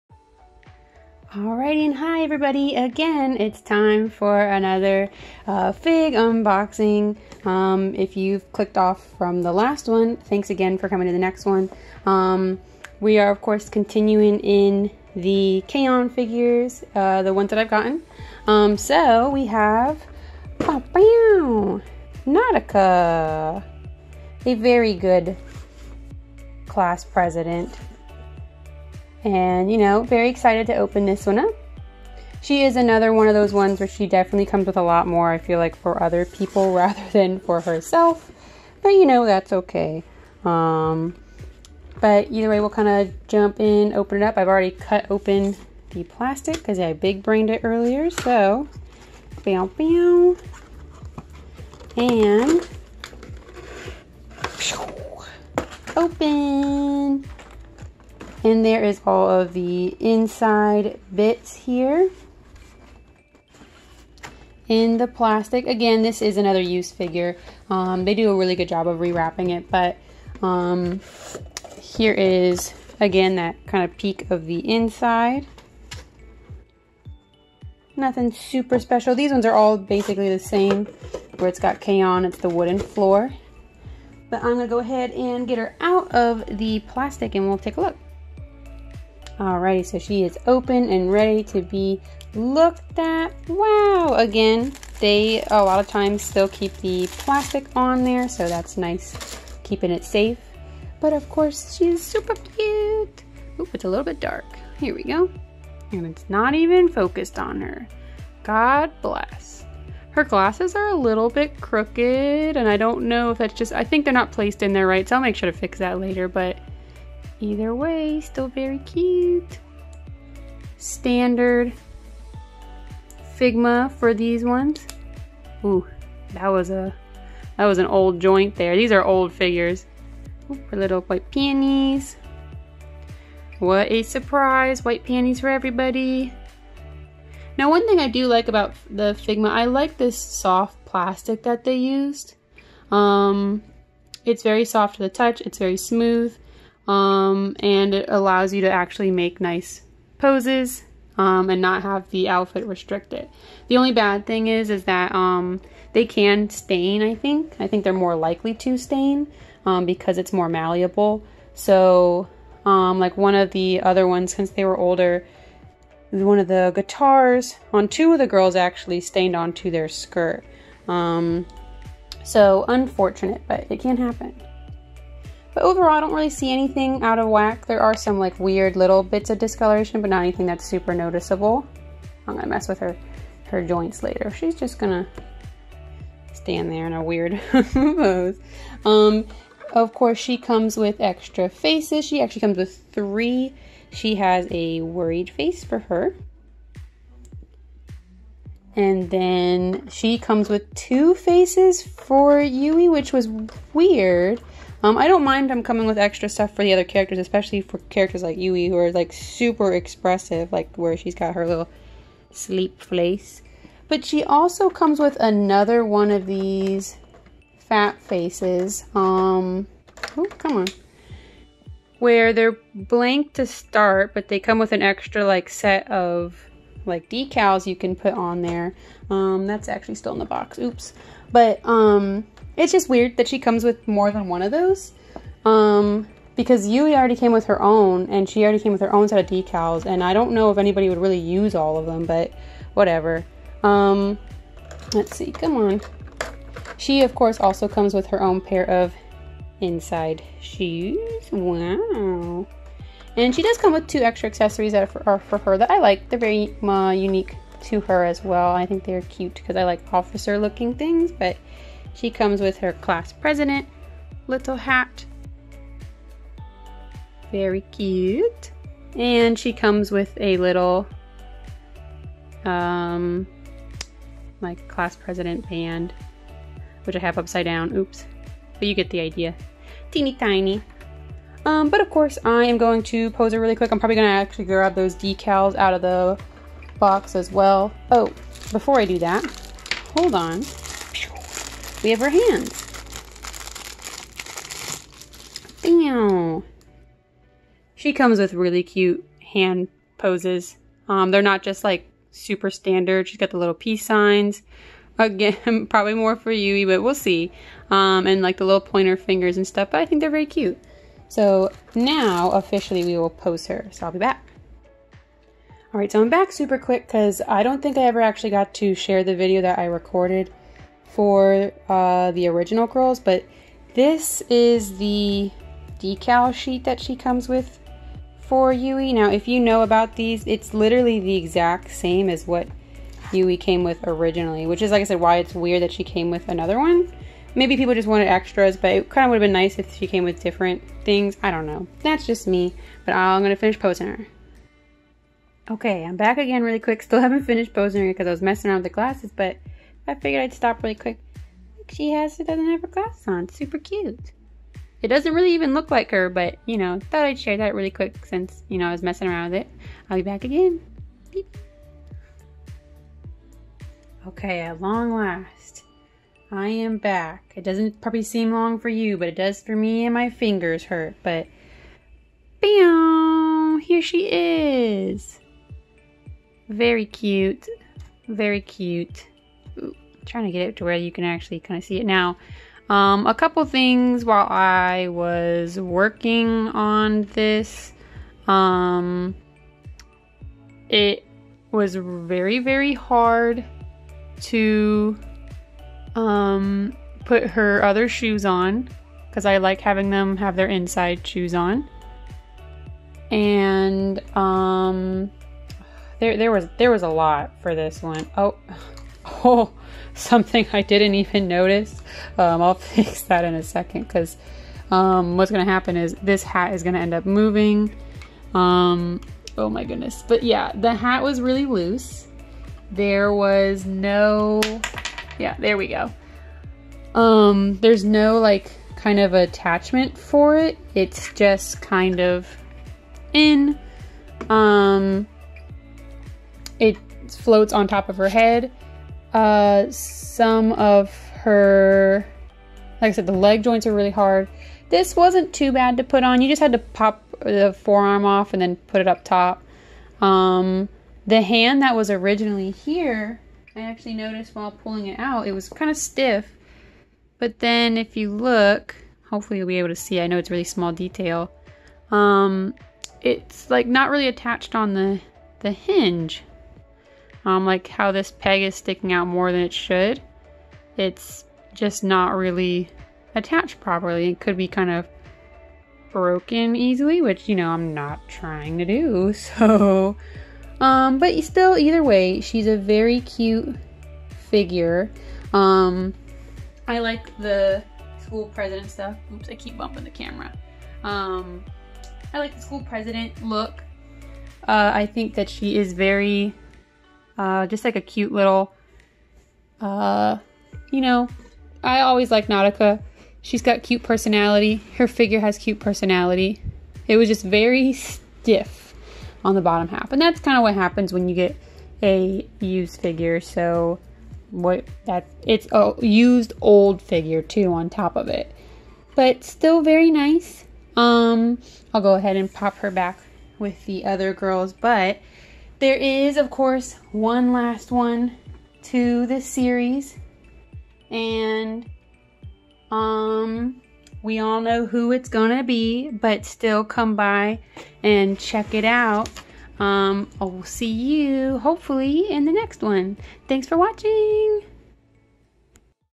Nadoka K-on Figma Review (Full Unboxing and Reactions~)
I continue to review the k-on figma I got for my birthday! Nadoka is a v cute addition for sure~

Thanks to my friend who helped edit the music for this vid you can follow them here: